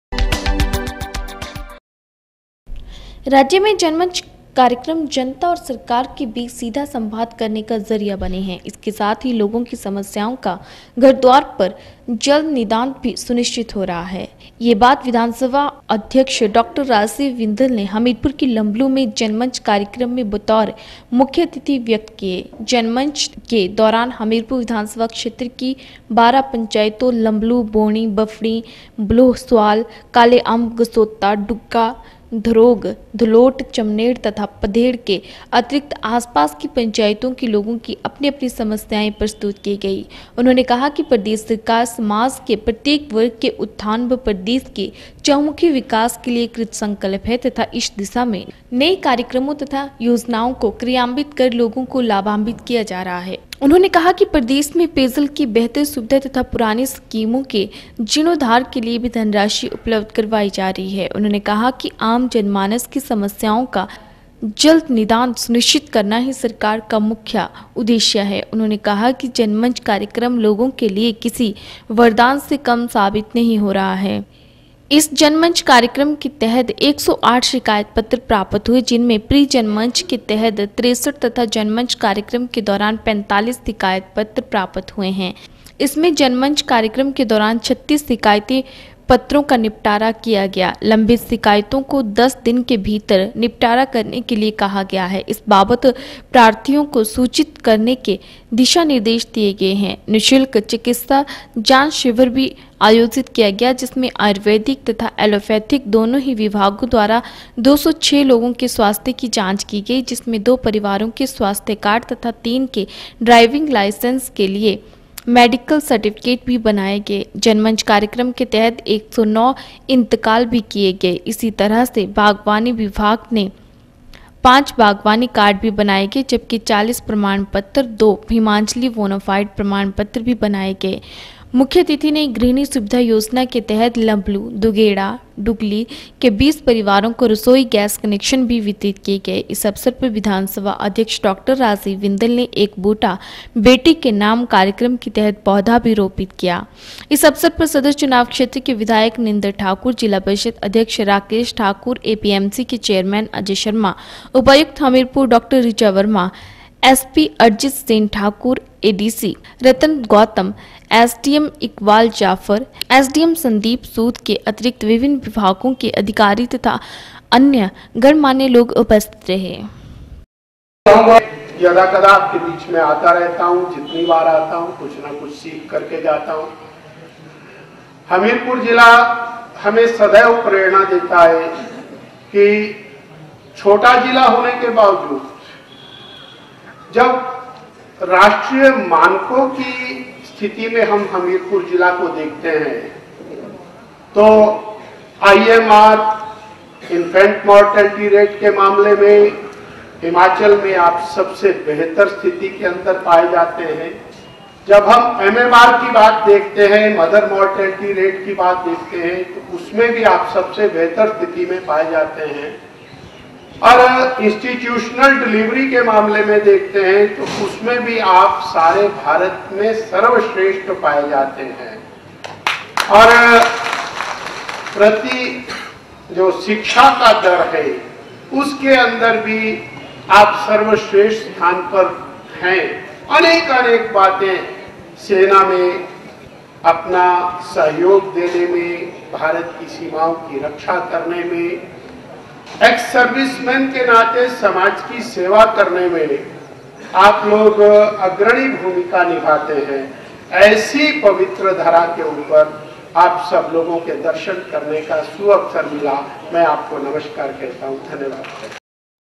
राज्य में जनता और सरकार के बीच सीधा संवाद करने का जरिया बना जनमंच कार्यक्रम
राज्य में जनमंच कार्यक्रम जनता और सरकार के बीच सीधा संवाद करने का जरिया बना है इसके साथ ही लोगों की समस्याओं का घर द्वार पर त्वरित निदान भी सुनिश्चित हो रहा है। यह उद्गार विधानसभा के अध्यक्ष डा राजीव बिंदल ने रविवार को हमीरपुर के लंबलू में जनमंच कार्यक्रम में बतौर मुख्यातिथि व्यक्त किए गए।